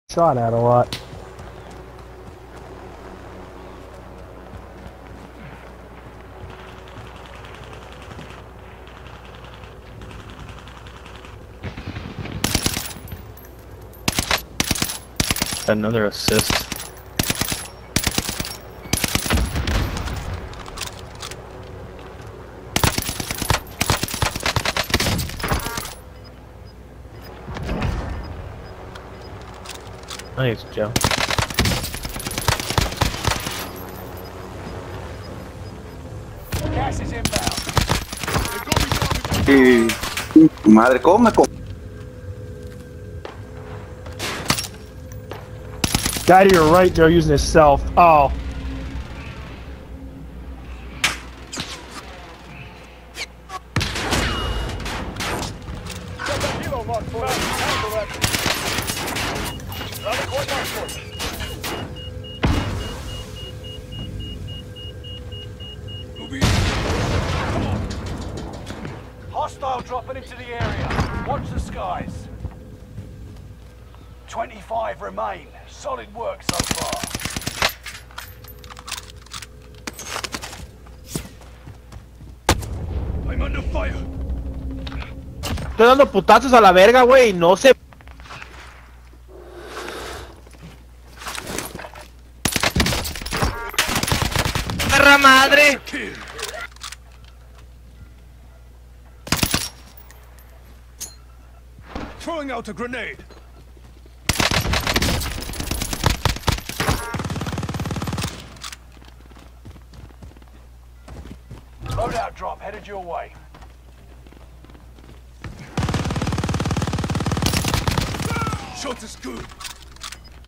Spanish Trash Talking (I think?)
This team did not like me, but I am basing that off of tone since I am not very fluent on my spanish.